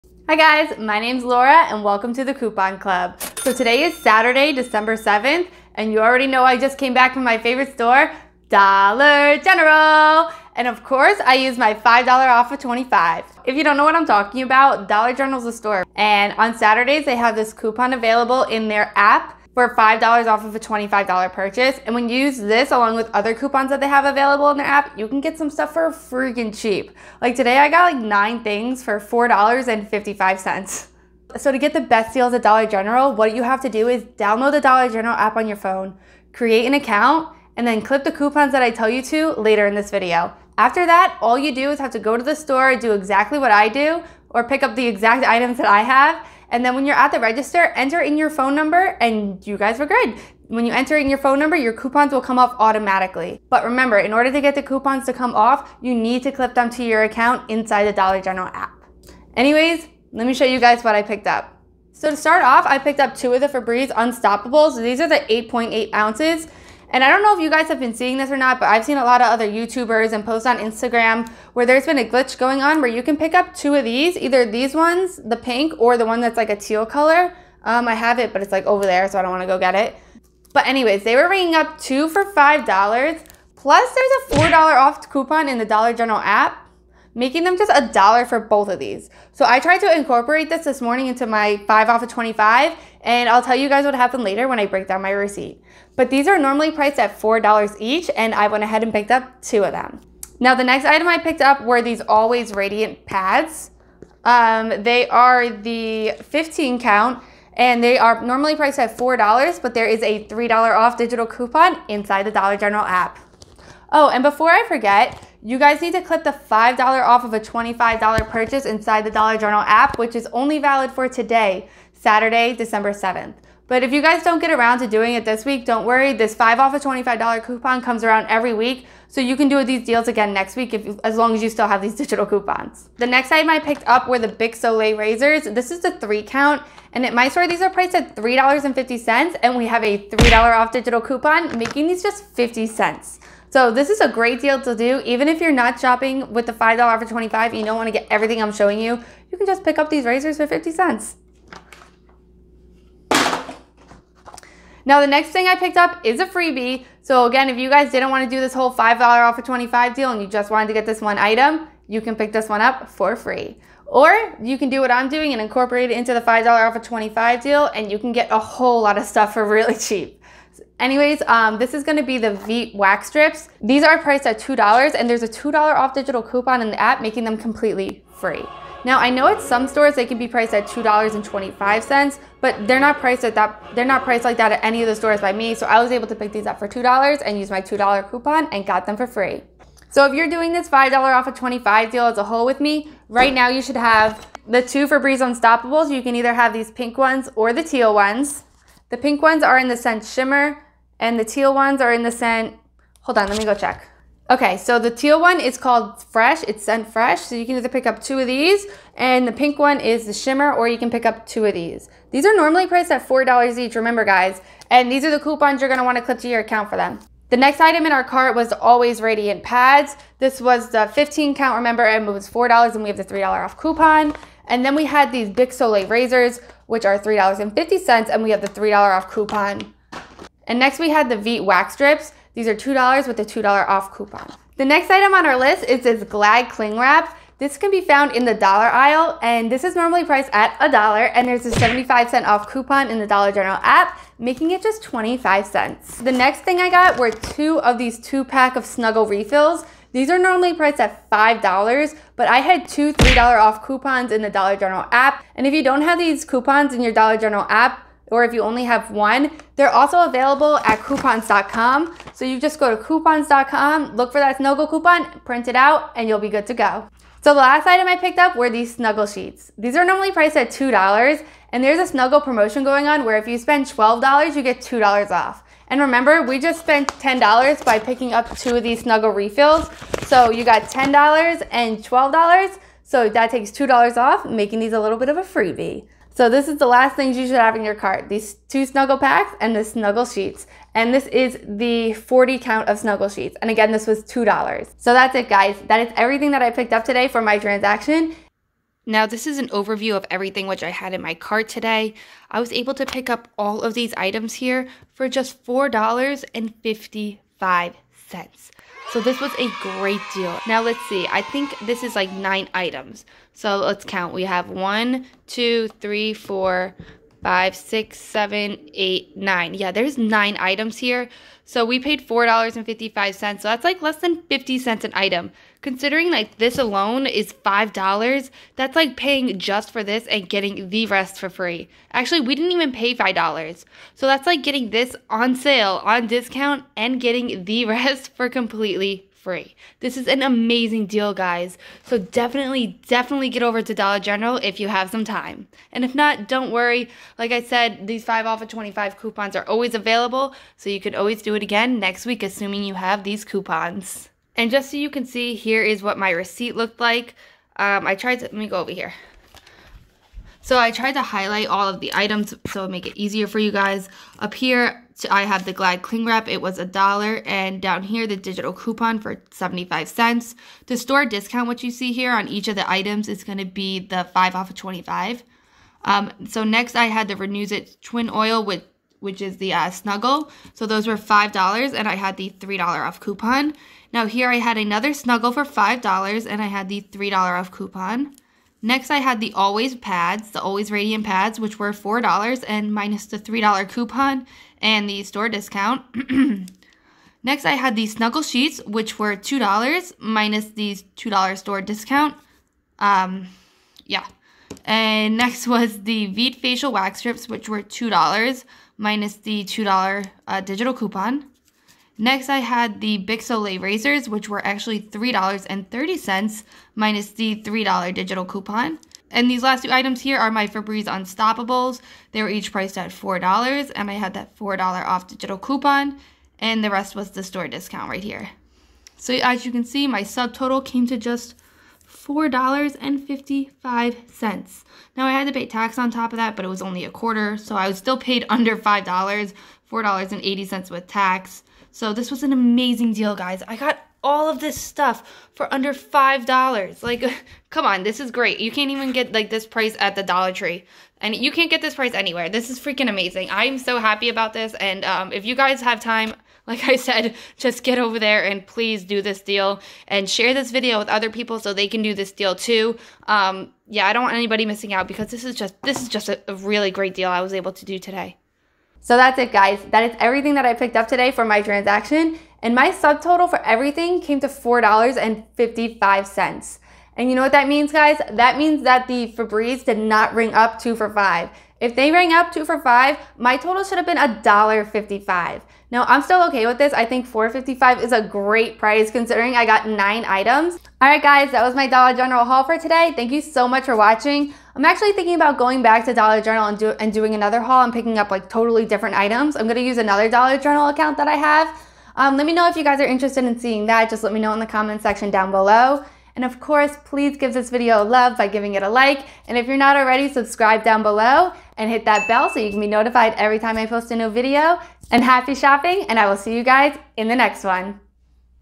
9 Items for UNDER $5 ~ Dollar General $5/$25 Deals ~ Amazing Deal!!!!
I tested the Febreze Glitch with the $5 off $25 at Dollar General! Does it work?? Watch until the end to find out

See how I was able to snag $9 items for under $5!!! Want to see me do it again with different items?

If you don’t have the $3 off Always Radiant Digital coupon, you can swap this out for the $3 Crest Toothpaste and use a $2 Crest Digital 😉